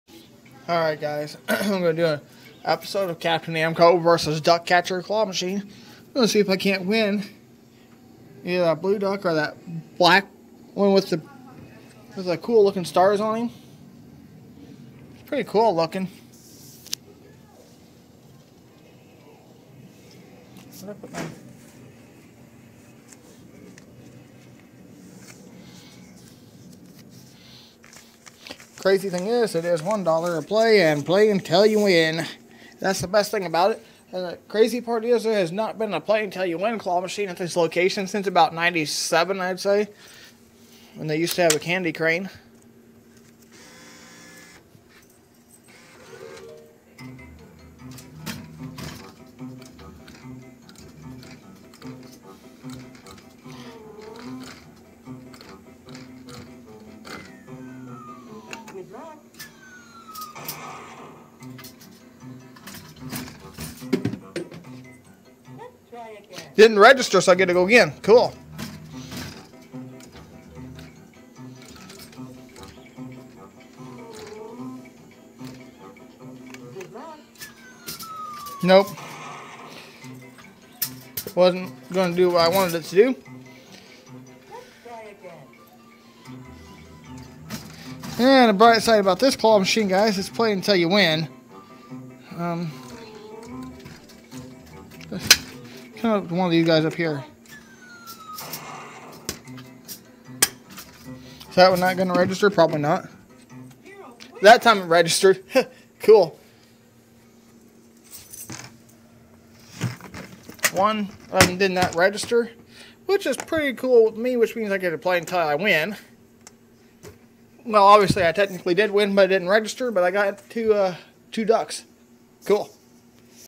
CAPTAIN NAMCO VS DUCK CATCHER CLAW MACHINE EPISODE 4 LETS TRY AGAIN
THIS WAS FILMED YESTERDAY EVENING AT LOCAL BOWLING ALLEY HRE I AM BACK AT THE DUCK CATCHER CLAW MACHINE FOR THIS EPISODE OF CAPTAIN NAMCO VS DUCK CATCHER CLAW MACHINE AS U WILL SEE 1 DUCK DIDNT REGISTER SO I GET TO GO AGAIN AND 2ND DUCK SET OFF THE PRIZE SENSOR